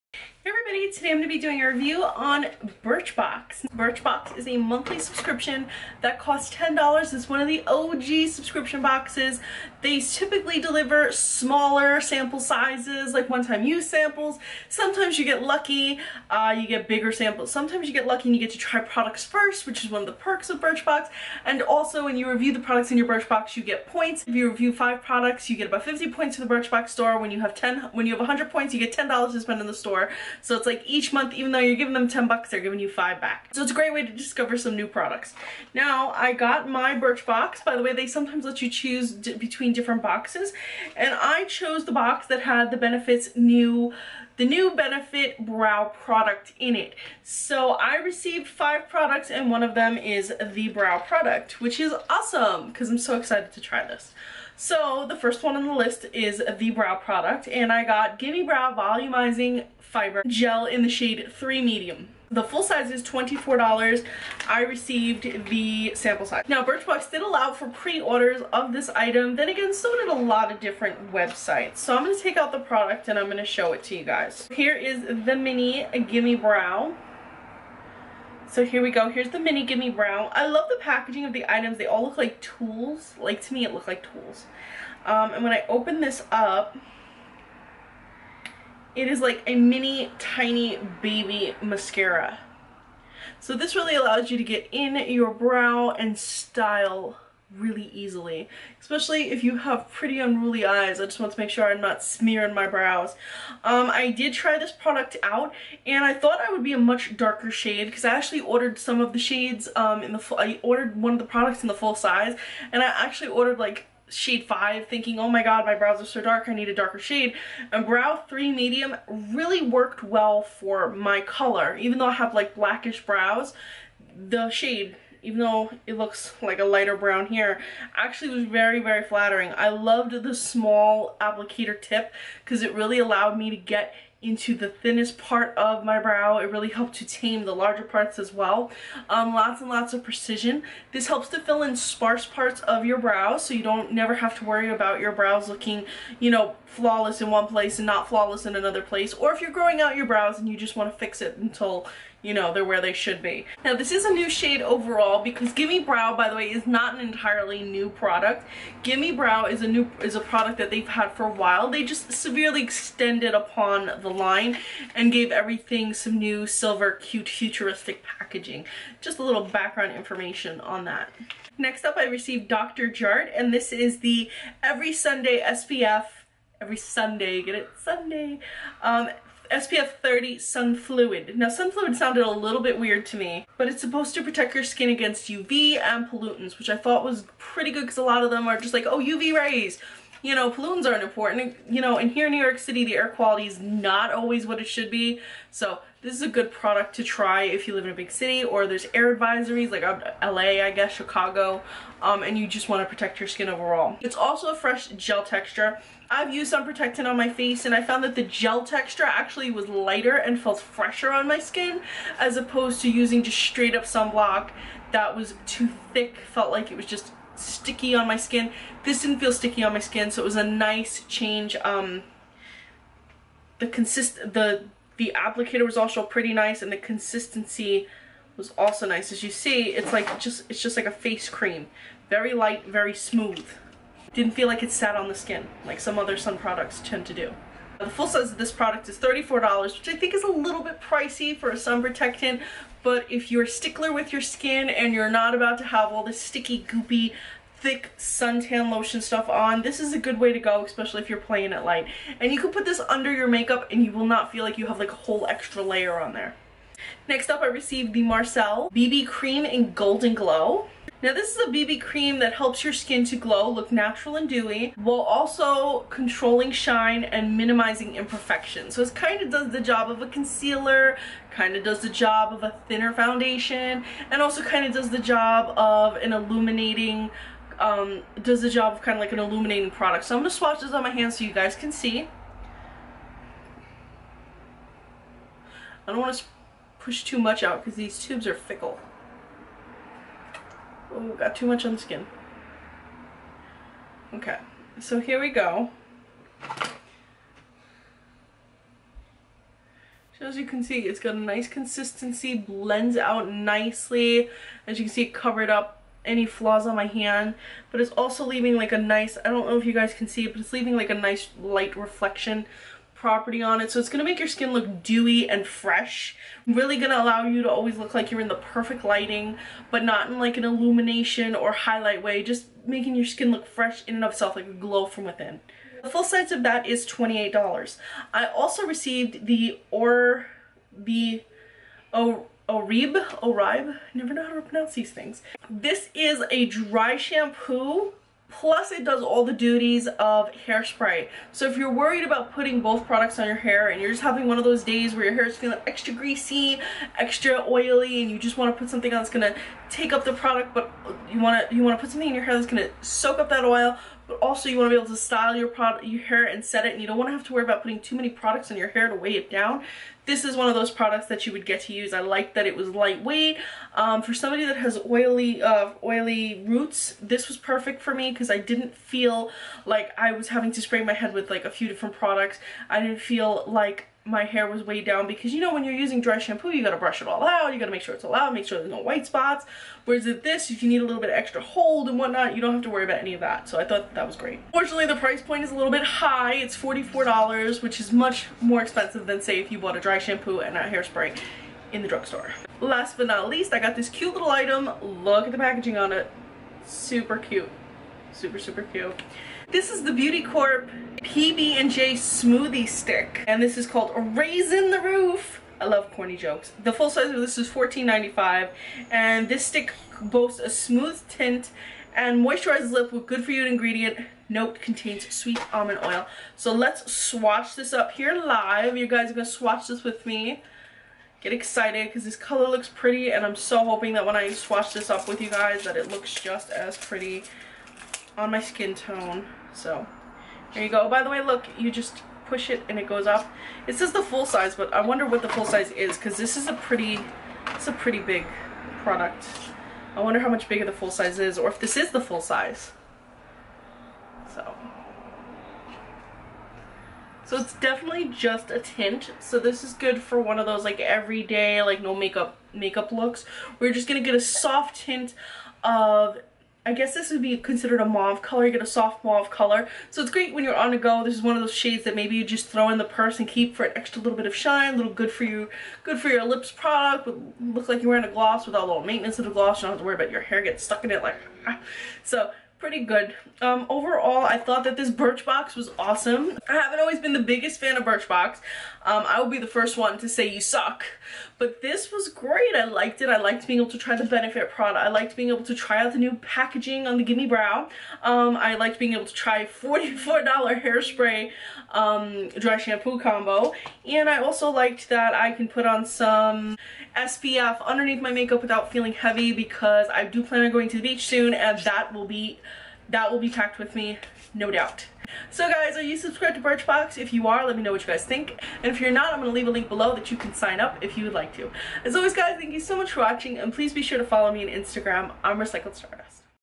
Birchbox Beauty Subscription: June 2016 review swatches
10$ monthly for beauty samples